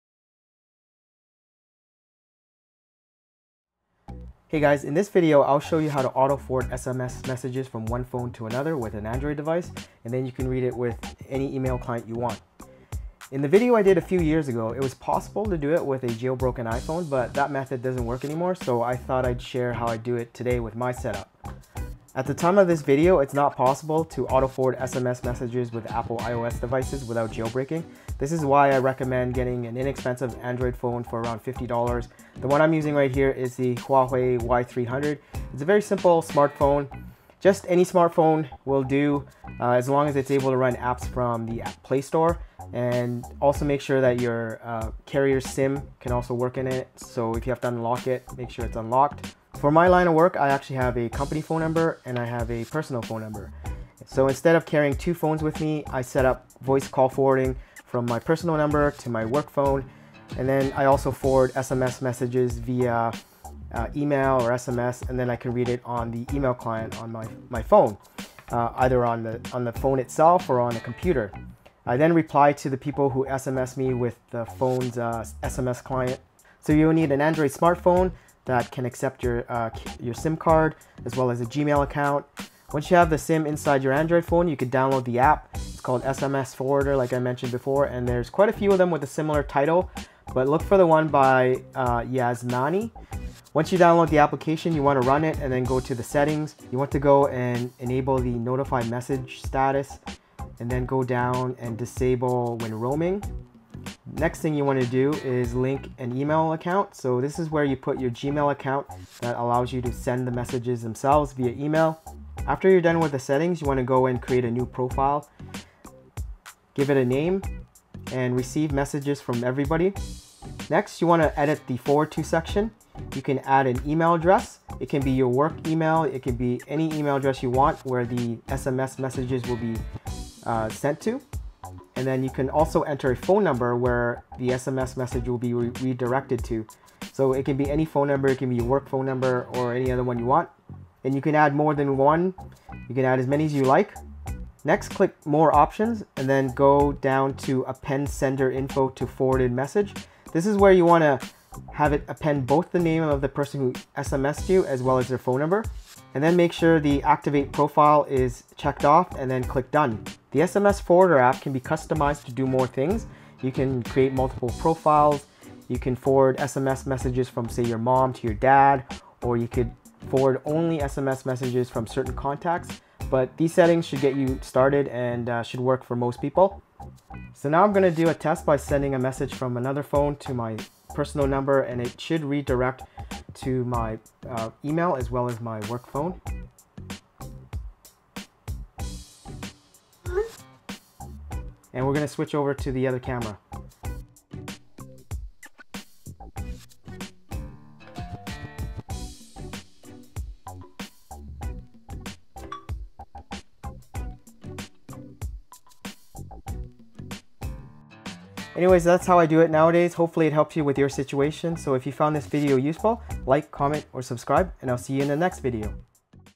Auto Forward SMS text messages (Android to iPhone) Updated
This is an updated version of a video I did back in 2013.  This is what I do today to get SMS forwarding.

In this video I will show you how to auto forward SMS messages from my Android phone to another phone (iPhone/Android/etc) using a free SMS forwarder app.  I go through the settings and also show a demo of it.  This is how I can forward personal SMS text messages to my work phone without having to carry both of them.  I also call forward the voice to the work cellular phone as well.  Any smartphone that can run Android apps should work, it doesn’t have to be the latest and greatest.  After setting it up, leave your personal phone plugged to power somewhere and you never have to worry about it.

I use to do iPhone to iPhone but iOS is very limiting in terms of allowing this so Android is the way to go if you want to do this.  Just find a cheap Android phone and install the app.  You’ll also need a Gmail.com or Yahoo.com account.

SMS Forwarder by Yasmani